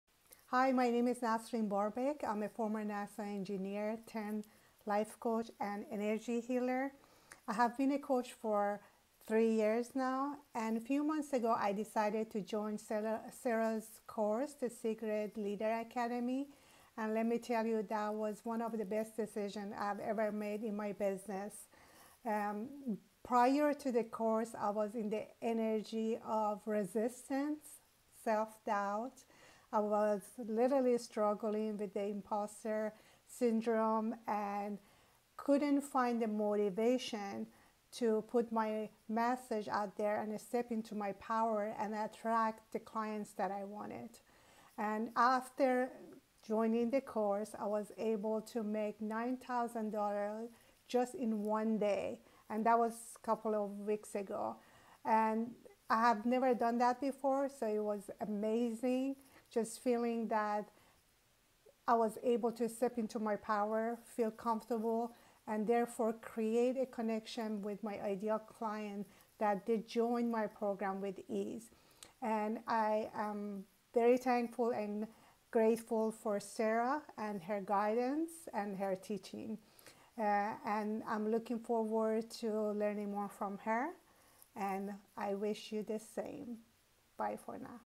🌟 Nasrin Generated 9K for her Spiritual Coaching Business After 3 Years of Struggling!
UPDATE: After filming this video Nasrin hit 18k in one week!

If Nasrin's journey resonates check out my FREE two-day mini-course exclusively for spiritual coaches, lightworkers, wayshowers, and new paradigm leaders!!

Namaste,

xx Sarah Rose
Divine Femme